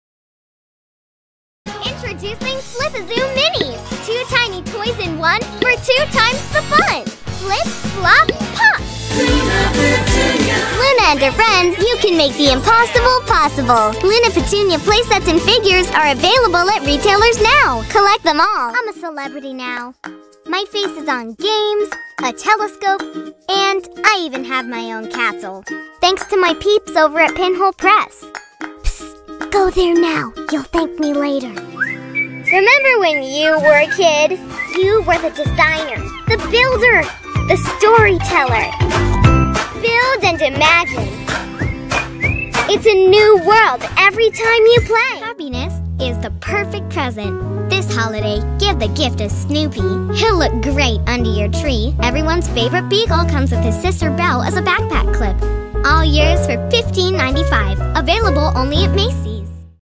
Glow Girls Kid Voiceover - Commercial Demo - Toy Industry
We love Toys! Glow Girls Kid Voiceover is the perfect choice to voice Toy commercials, sing product Jingles, and even bring a Talking Toy to life!